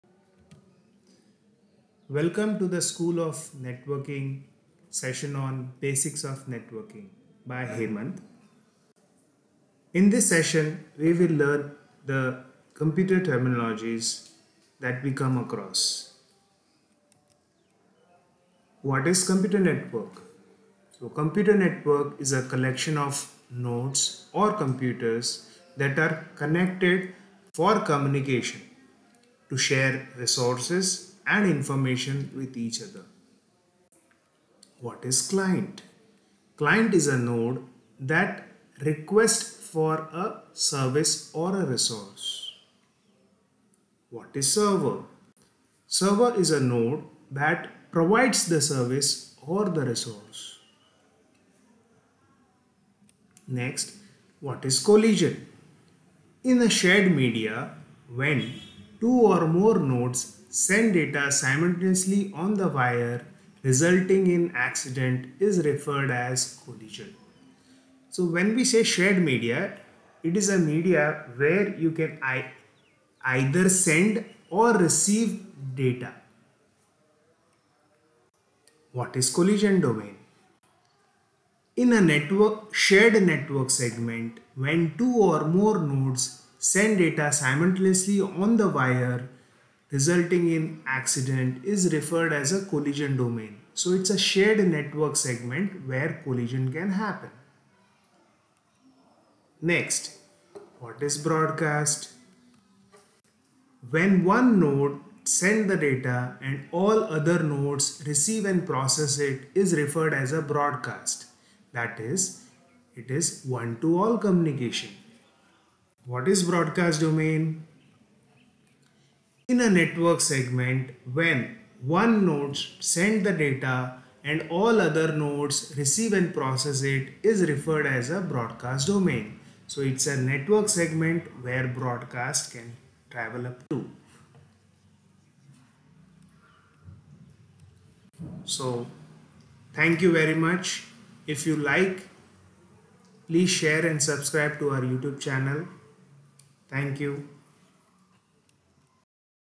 Lesson 01 Networking Terminology
Computer Networking - Terminology
Computer Network, Client, Server, Collision, Collision Domain, Broadcast and Broadcast Domain.